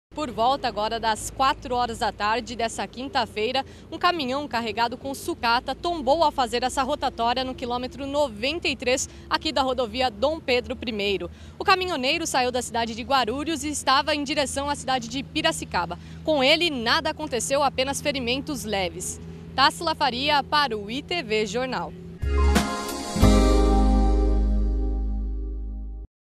Carreta de sucatas tomba na Rod. D. Pedro I, em Itatiba
Diário i | O Portal de Notícias da ITV Brasil.